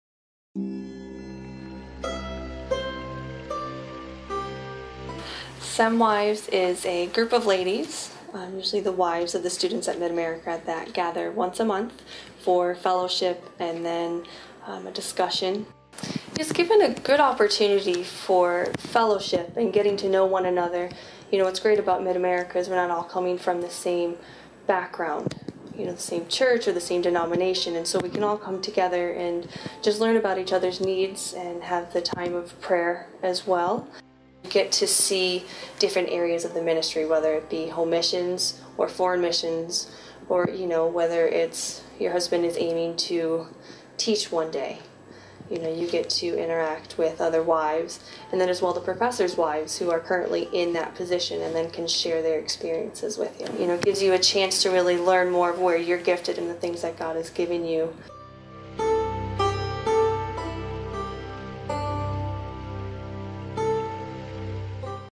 Life at MARS: Women's Fellowship
Women's fellowship is a chance for students' and professors' wives to gather together to fellowship, study, and encourage one another.